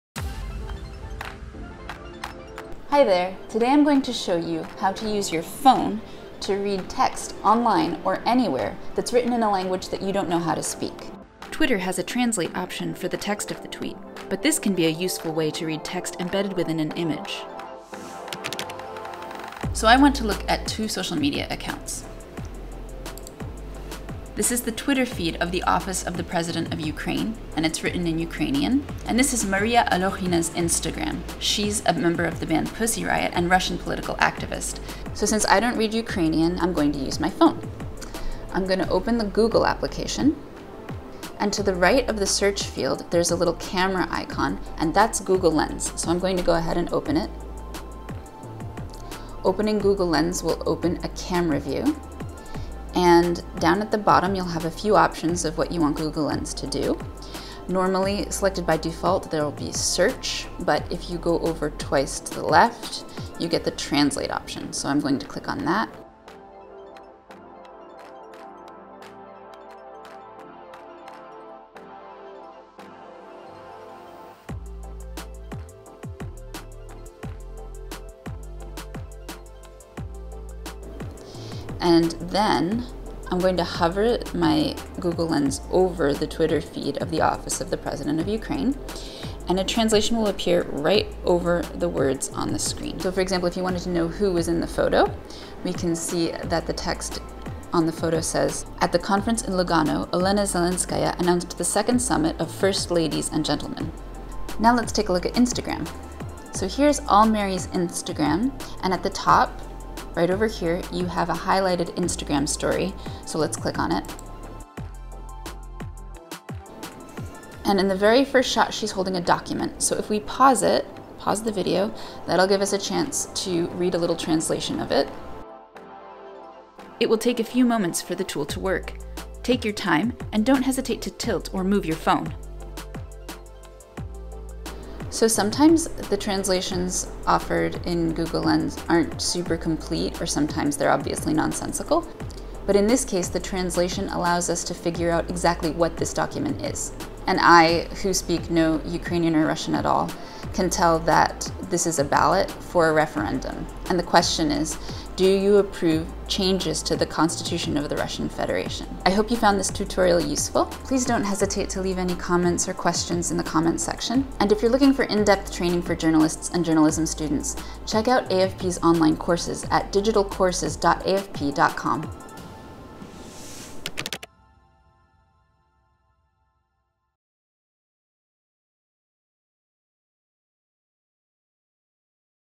How to use your smartphone to read foreign languages
Ever been stuck with text in a language you don’t speak and that can't be translated by your browser? Your smartphone is here to help!

In this short video see how you can use your phone's camera to decipher protest signs, memes and embedded text in other languages.

See more quick tutorials on the Online Investigation Tips for Journalists Youtube playlist

Disover more in-depth explanations of digital investigation techniques, with our digital courses